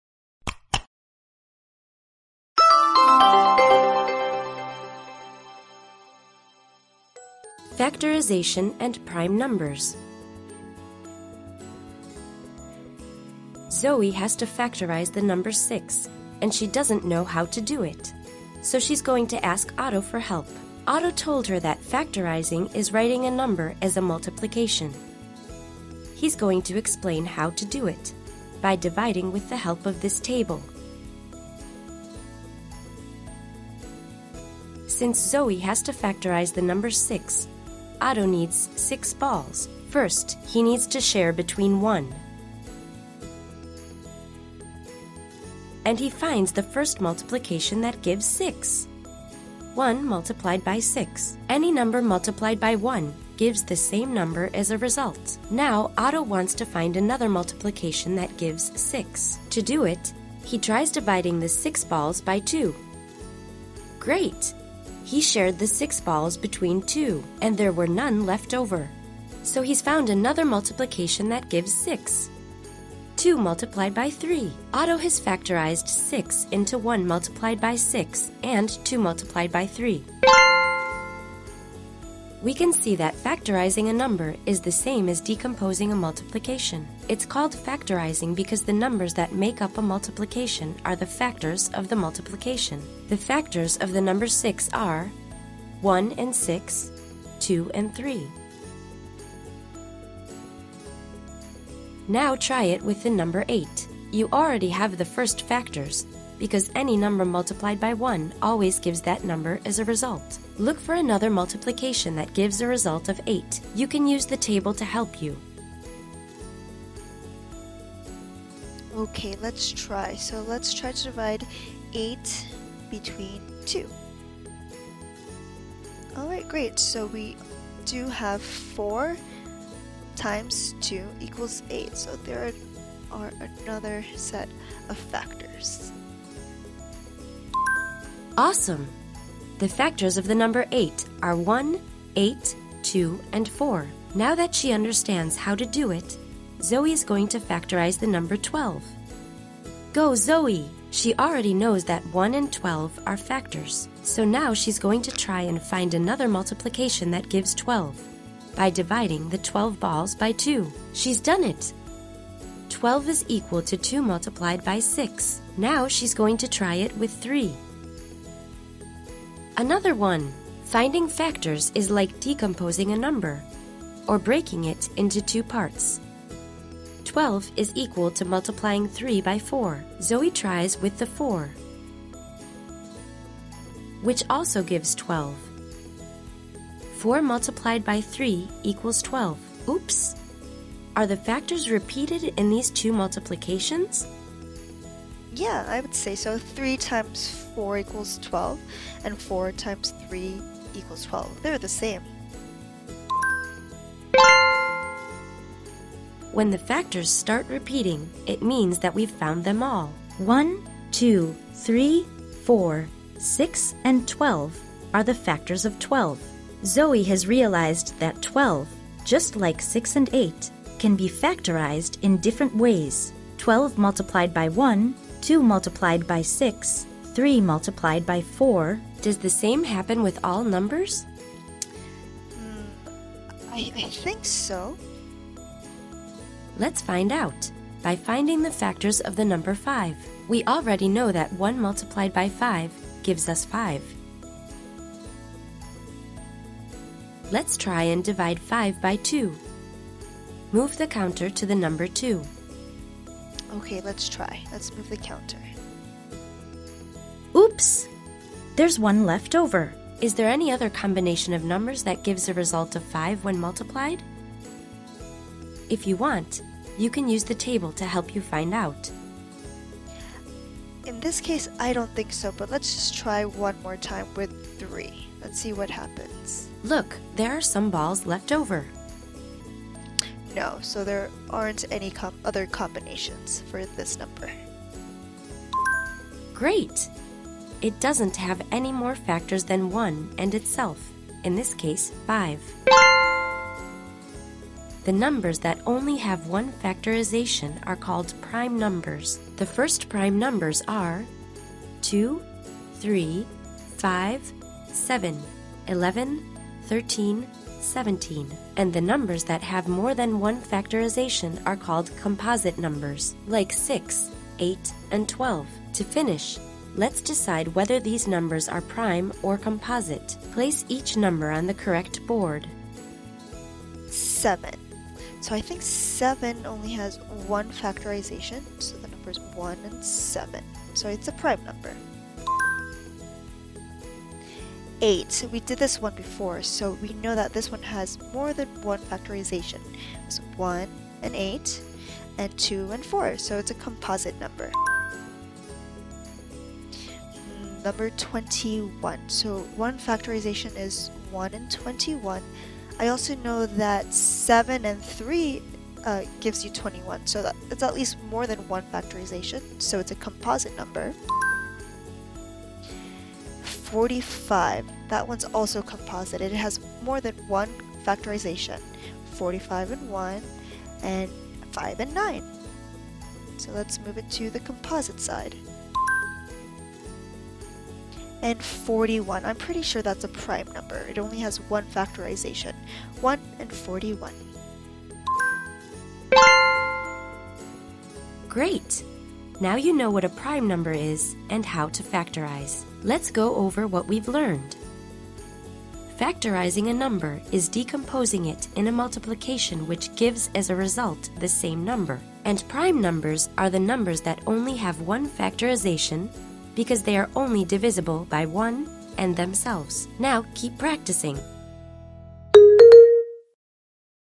Step-by-Step Factorization and Prime Numbers - Smartick
This short tutorial reviews factorization and prime numbers and demonstrates how to factorize numbers with help from the Montessori table. You will also learn about prime and composite numbers.
Do you have any questions about this tutorial? Leave your questions in the comments.